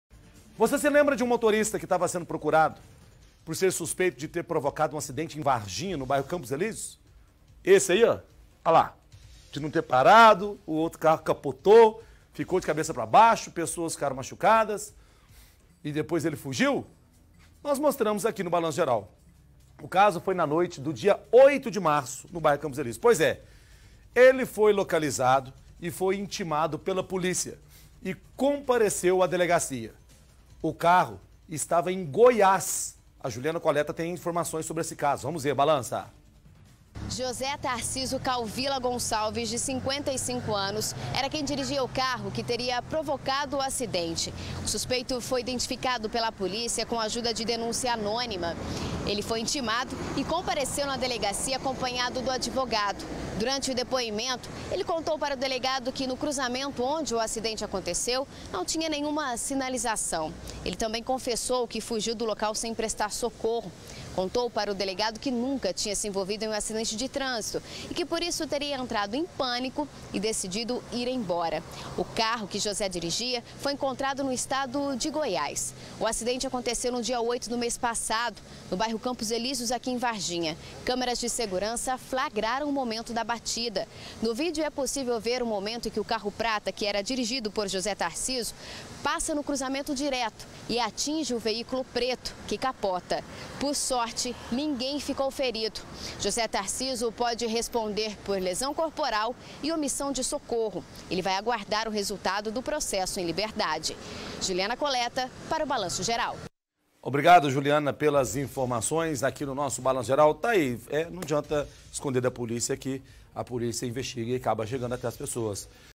Varginha: motorista fujão é encontrado pela polícia
Foi identificado o motorista que estava sendo procurado por ser suspeito de ter provocado um acidente em Varginha. O caso foi na noite do dia 08 de março, no bairro Campos Elíseos. Pois ele foi localizado, intimado pela polícia e compareceu à delegacia. O carro estava em Goiás.